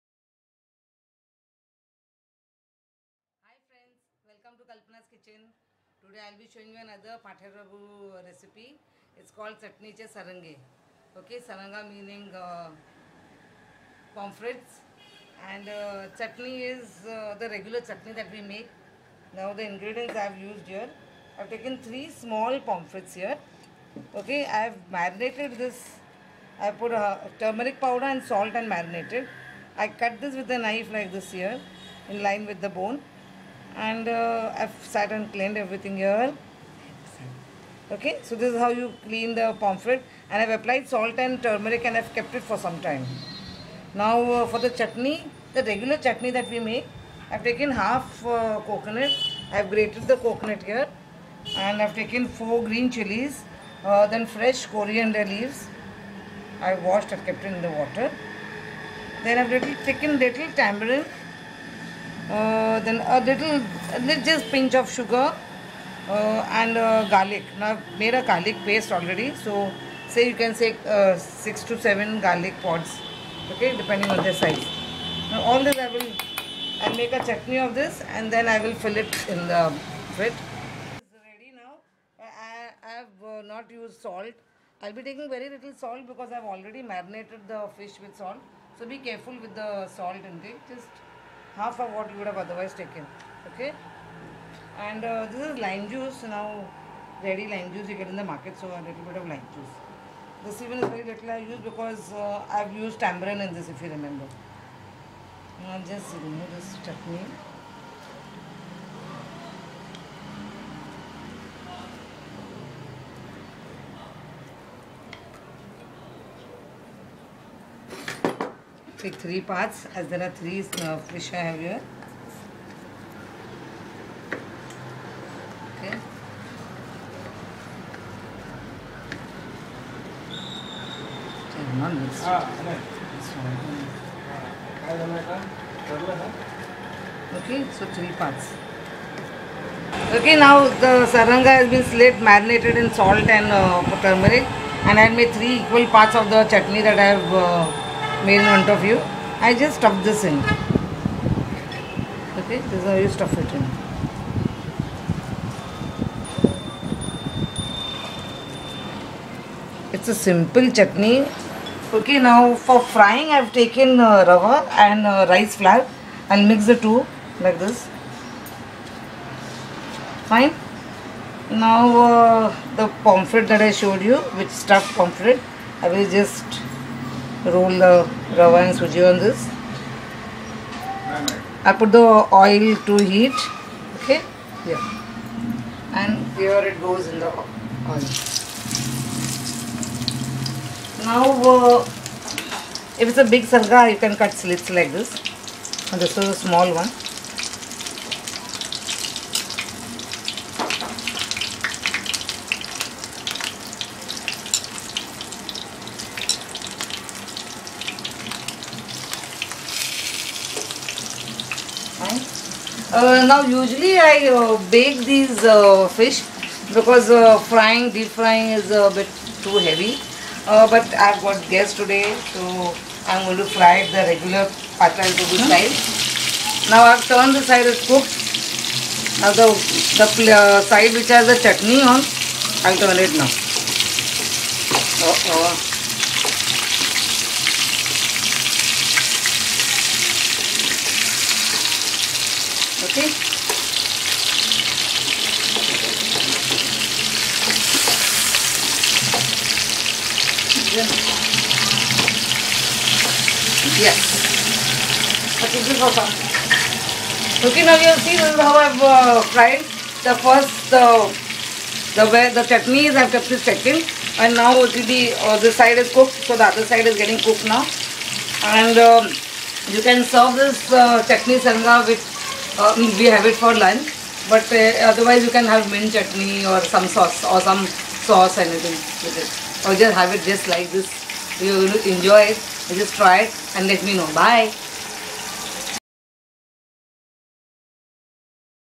Fried Stuffed Pomfrets | Chutney che Sarange By Kalpana Talpade
Stuffed Pomfrets / चटणी चा सरंगा / Chutney che Sarange By Kalpana Talpade

Silver Pomfrets stuffed with Chutney, fried Pathare Prabhu style.

Recipe:
Ingredients : Pomfrets (small) 3
Fresh Coconut  1/2
Garlic   7
 Fresh coriander leaves  1 bunch
Tamarind  little
Sugar   little
Water                   1 cup
Lime juice  1 tblsp.
Rice flour  2 tblsp.
Rawa   2 tblsp.